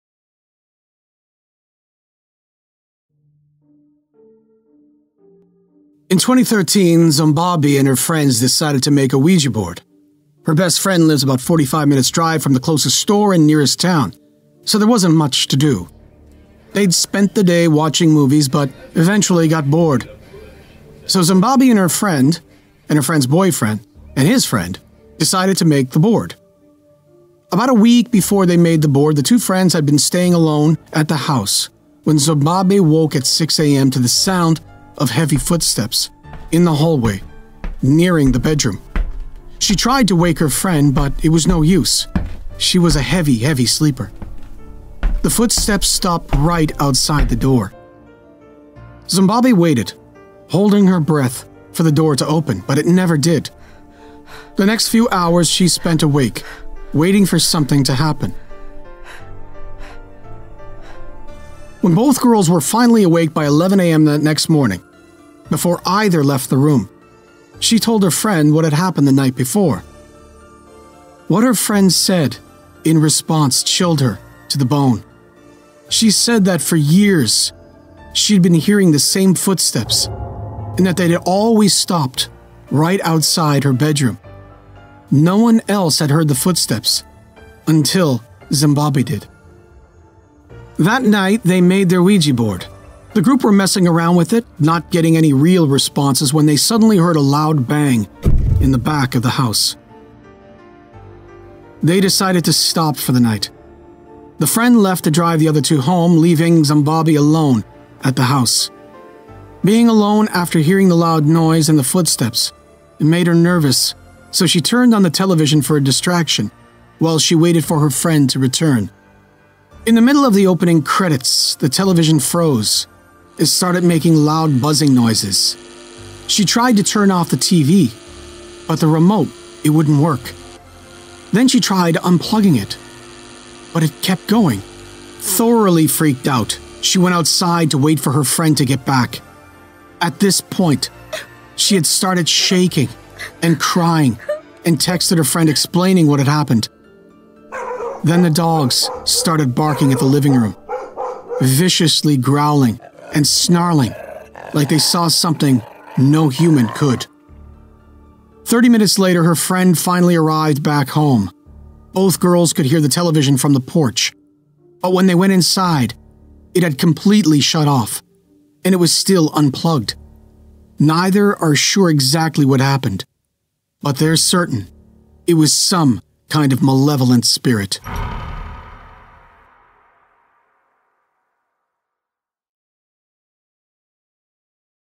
Threatening Footsteps by Reddit User Xombabe
Noises in the night. enough is enough.

A Short Creepy Tale Written by Reddit User Xombabe